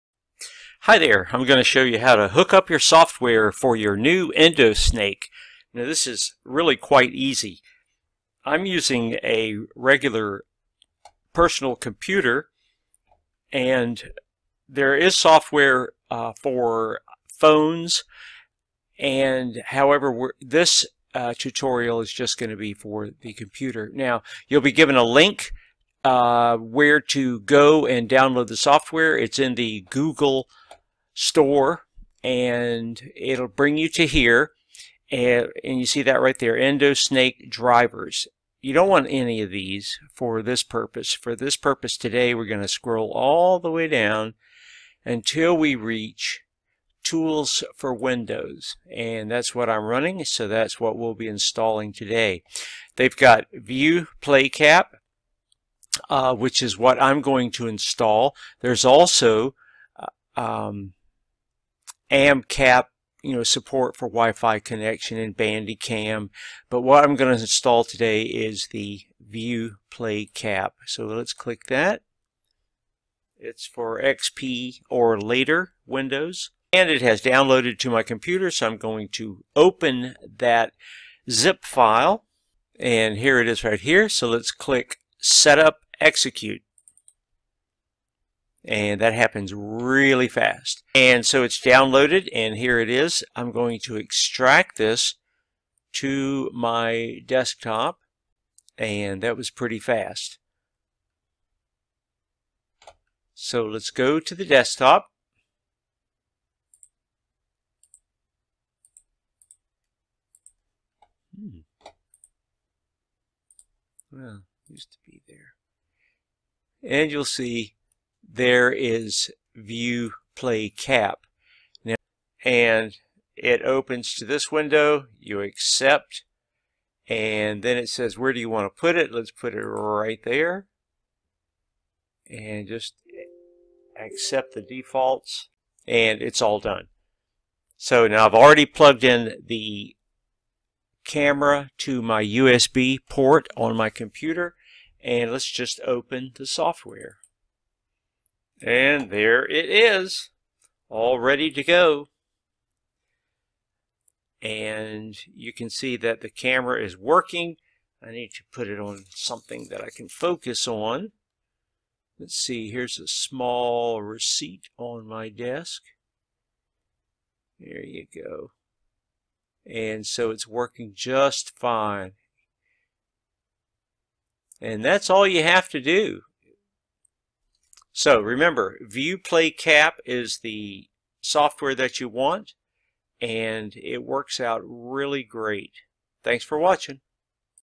How to use EndoSnake Viper 3.9mm with PC computers
A detailed user review and how-to video  instructions of the EndoSnake Viper 3.9 borescope with WiFi